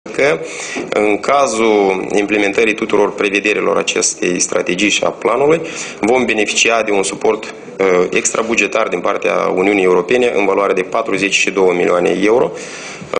Declarațiile lui Alexandru Jizdan după ședința Guvernului Republicii Moldova din...
42 milioane euro. Cu at&#226;t se pare că este v&#226;ndută libertatea noastră online!
Titlul evenimentului: Declarațiile lui Alexandru Jizdan după ședința Guvernului Republicii Moldova din 30 martie 2016